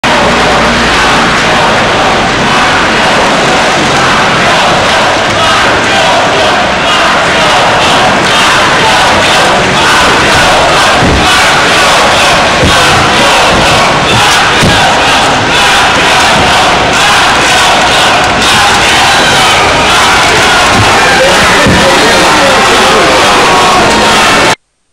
Berlusconi accolto ad Helsinki al grido di "mafioso"
4 Marzo 2011 - Berlusconi viene accolto nella capitale finlandese al grido di "mafioso"